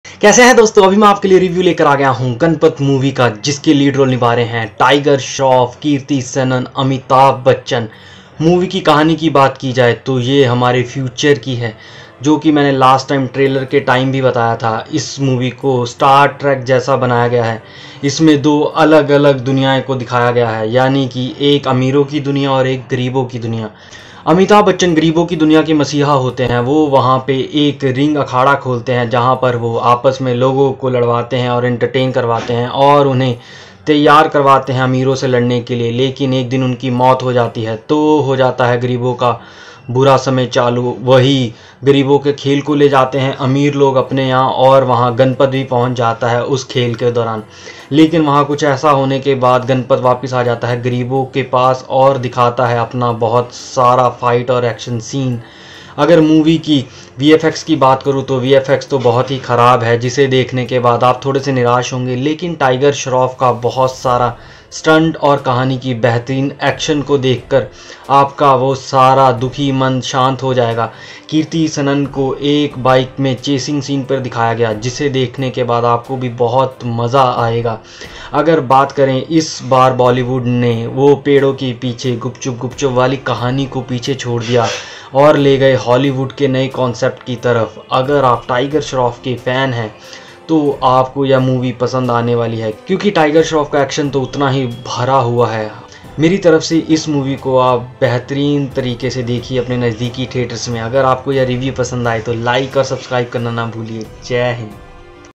Ganapath movie review
Ganpat movie is bar to Bollywood mein kuchh alag hi kar dikhaya shukar hai yah Laila Majnu ke pyar se bahar to aap aaye aur audience ke liye Hollywood ki tarah kuchh naya kaun